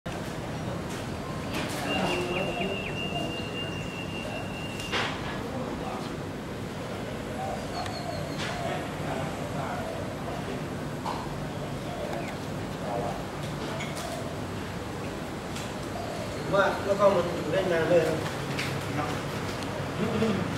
59 Che Mai Cham Che, Lang Mo เจไม่จำเจ หลังมอ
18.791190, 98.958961
14/1 Mu 8, Suthep Road, Chiang Mai 50200, Thailand. Behind Chiang Mai University. Open every day 11:00-23:00. Since 2013; moved in 2016. Price: 30-50 baht.

14/1 หมู่ 8 ถนนสุเทพ ตำบลสุเทพ อำเภอเมืองเชียงใหม่ จังหวัดเชียงใหม่ 50200 หลังมหาวิทยาลัยเชียงใหม่ เปิดทุกวัน 11:00-23:00 น. ตั้งแต่ปี 2556; ย้ายร้านปี 2559 ราคา 30-50 บาท โทรศัพท์ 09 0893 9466

Video: 20 October 2016

by Chiang Mai Vegetarian Project
โดย โครงการมังสวิรัติเชียงใหม่